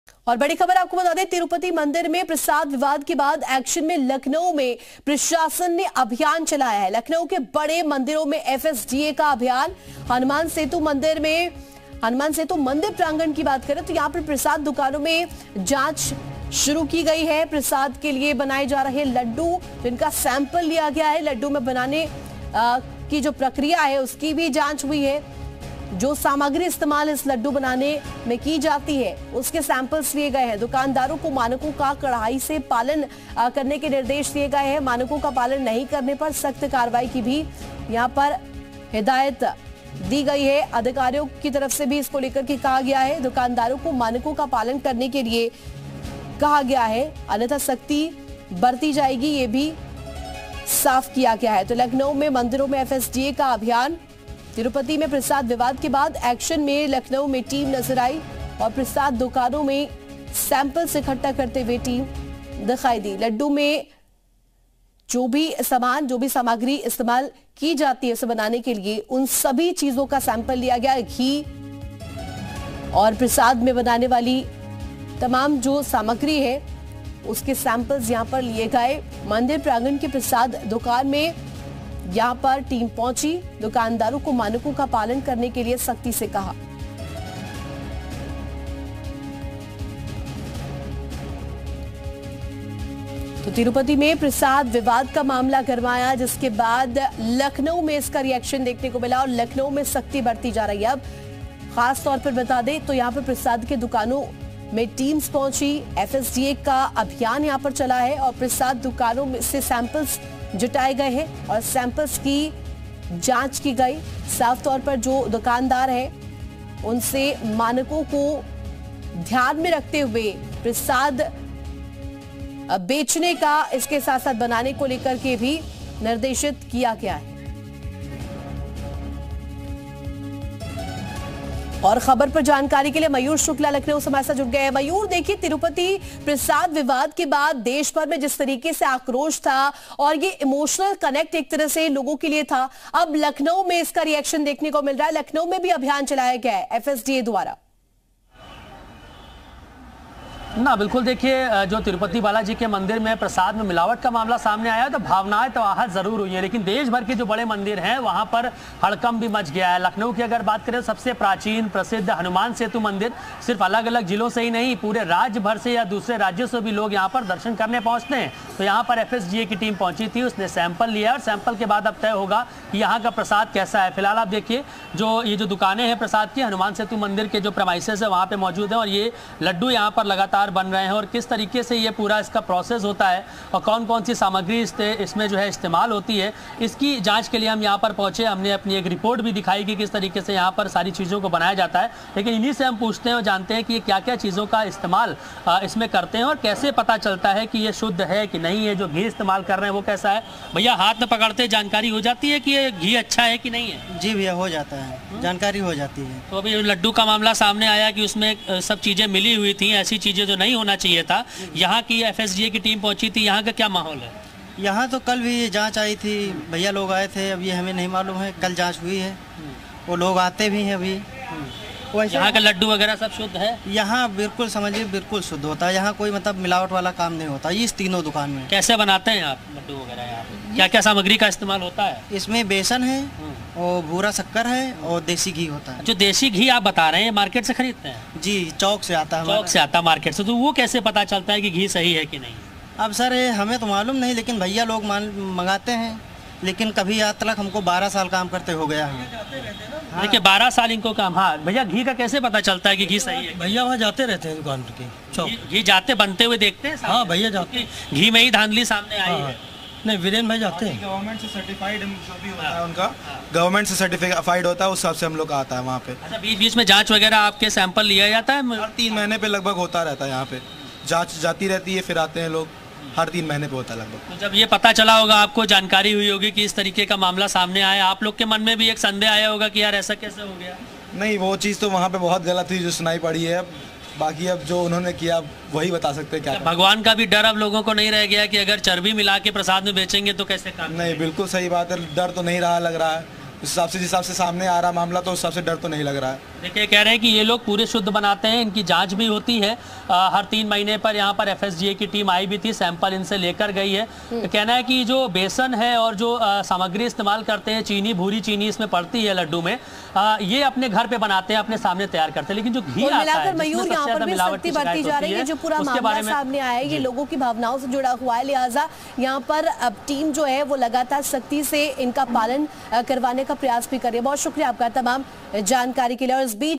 Breaking News: तिरुपति विवाद के बाद लखनऊ में एक्शन शुरू | Animal Fat | Charbi | Tirupati Prasad
Lucknow : तिरुपति मंदिर में प्रसाद विवाद के बाद एक्शन, लखनऊ के मंदिरों में FSDA का अभियान, हनुमान सेतु मंदिर प्रांगण में जांच अभियान,मंदिर प्रांगण की प्रसाद दुकान से सैंपल लिए,दुकानदारों को मानकों का पालन करने को कहा

Lucknow: Action after Prasad controversy in Tirupati temple, FSDA campaign in temples of Lucknow, investigation campaign in Hanuman Setu temple premises, samples taken from Prasad shop in temple premises, shopkeepers asked to follow the standards.